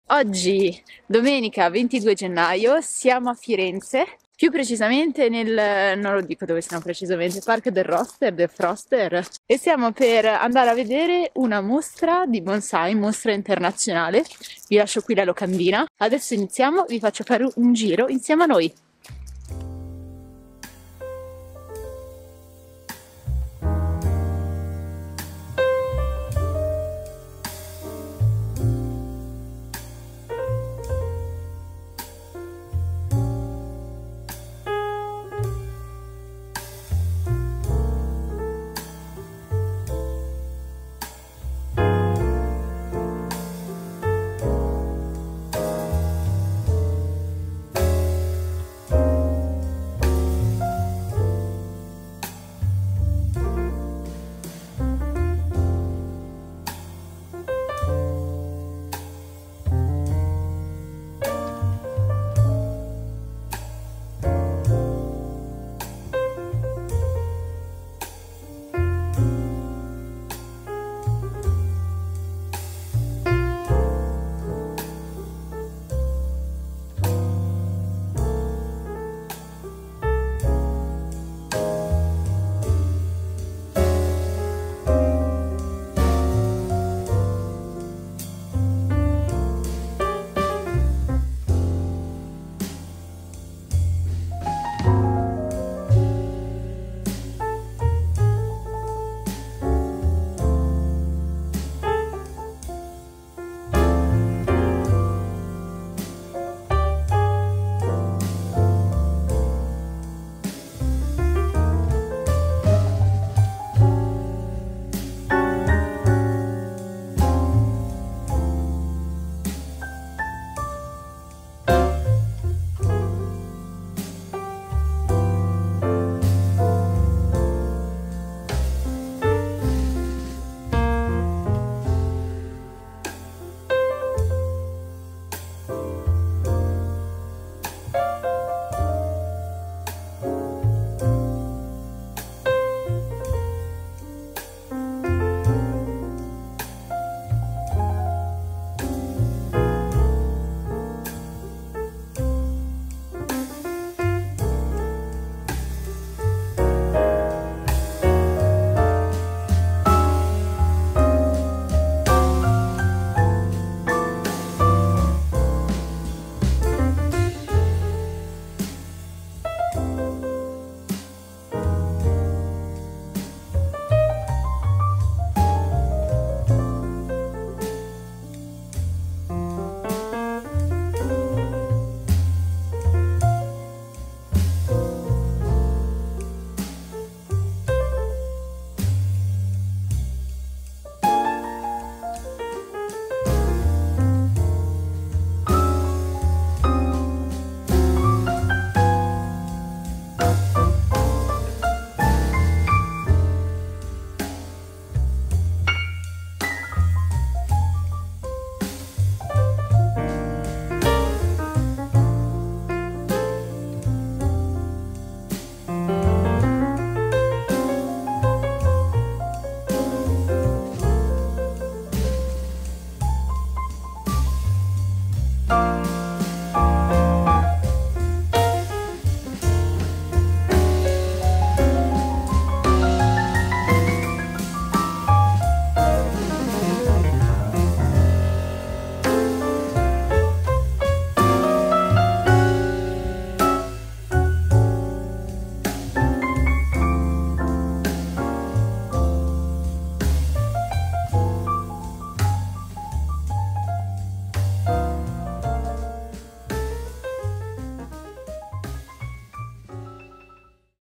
Una SERRA piena di Bonsai / NATURA DI INVERNO A FIRENZE
un'esposizione di Bonsai a Firenze in una location a dir poco spettacolare.
Ne siamo rimasti affascinati e spero che dal video si possa capire la bellezza delle piante e dei Suiseki portati in mostra.
Complimenti agli organizzatori e agli espositori!